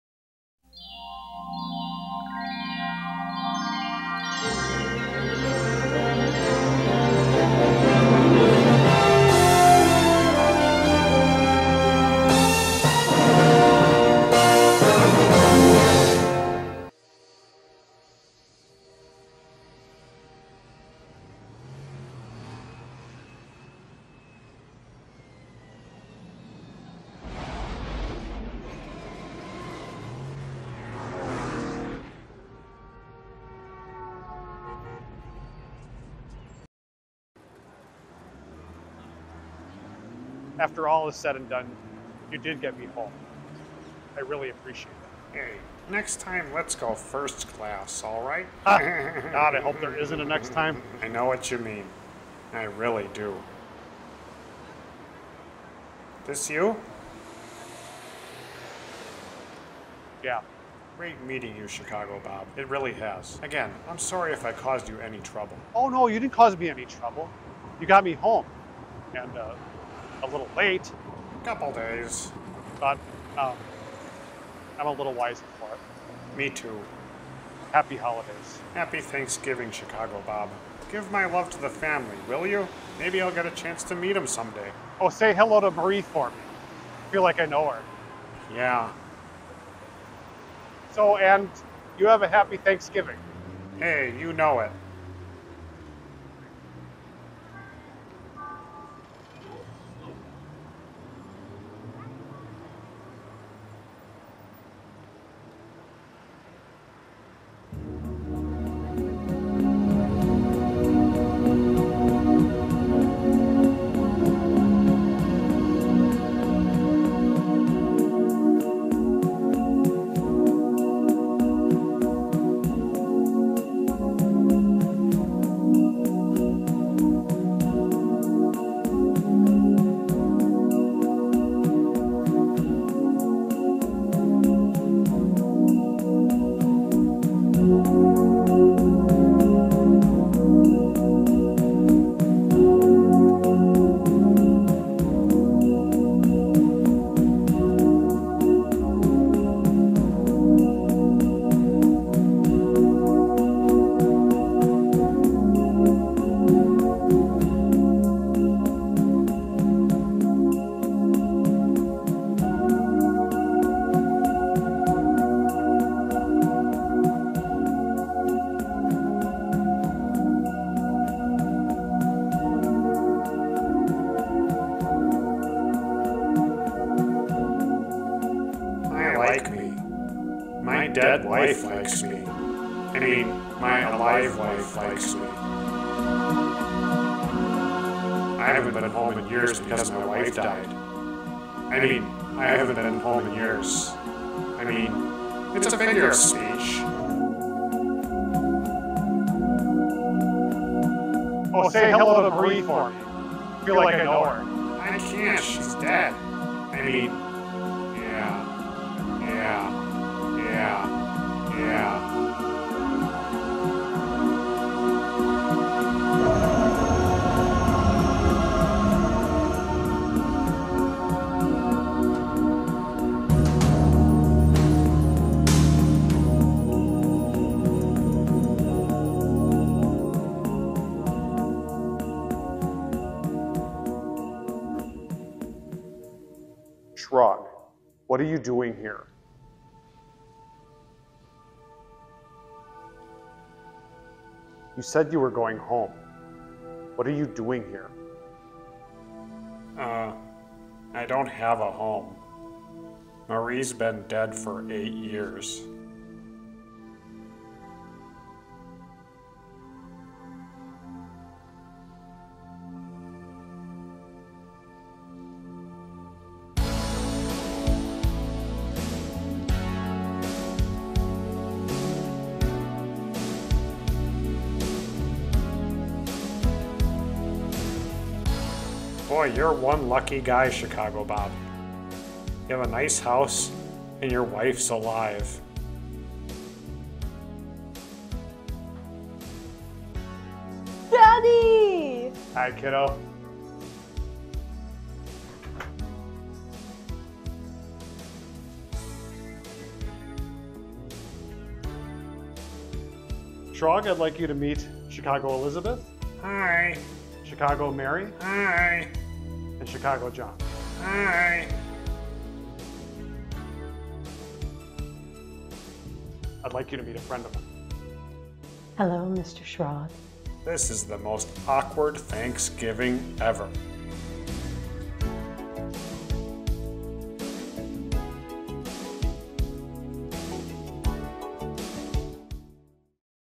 Planes, Trains, & Chicago Bob
For such a funny movie, Planes, Trains, and Automobiles had a really sad ending. In this special edition of Chicago Bob, Shrog and I recreate the ending using all of the movie’s Chicagoland filming locations.
The El Track, the train station, and the mansion are all used for this not-so-sad ending to a classic comedy.